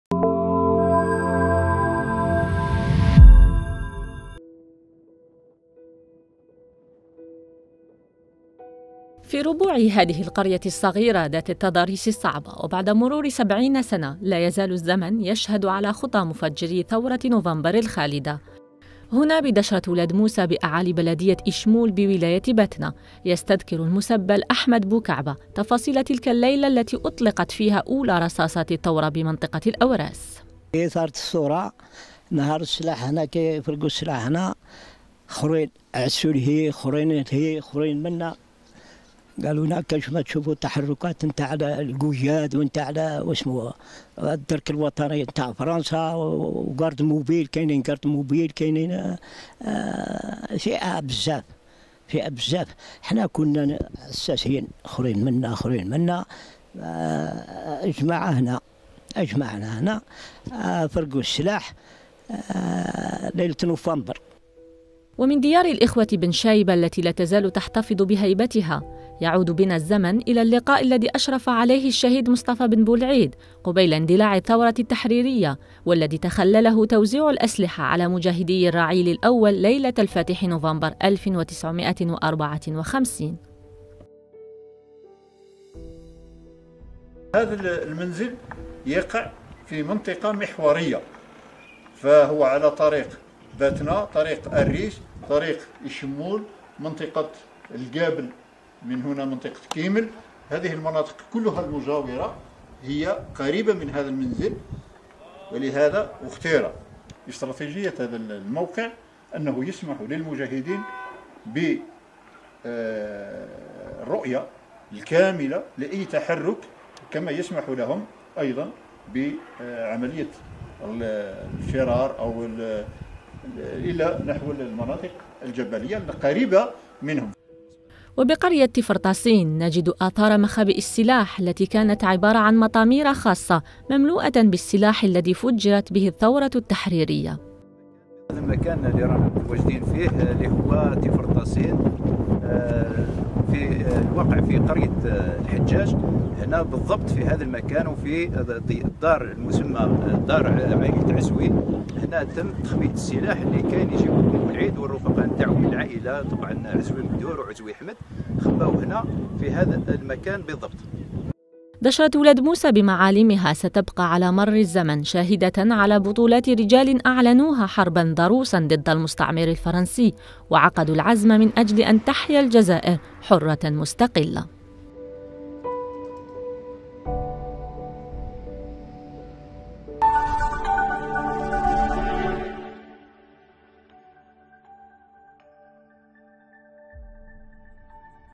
دشرة أولاد موسى بالأوراس.. شاهد على عظمة ثورة وتاريخ أمة
قسنطينة(وأج) - وأنت تتجول بدشرة أولاد موسى بولاية باتنة، تعود بك الذاكرة،  إلى سبعين سنة خلت وتقرأ في كل شبر من هذه الأرض الطاهرة ، حكاية شعب انتفض ذات نوفمبر، ليقول للعالم بصوت واحد لا للمستعمر.. لا للمحتل..حيثما وليت وجهك، ثمة فصل من فصول التاريخ الذي صنعه رجال آمنوا بأن للحرية الحمراء باب بكل يد مضرجة يدق.